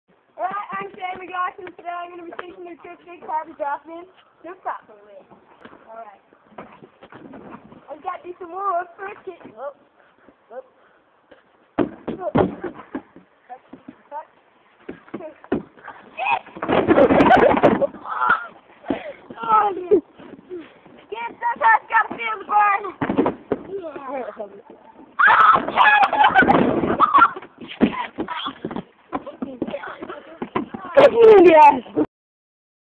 how to drop in with shane
how to drop in with shane and his epic board to the head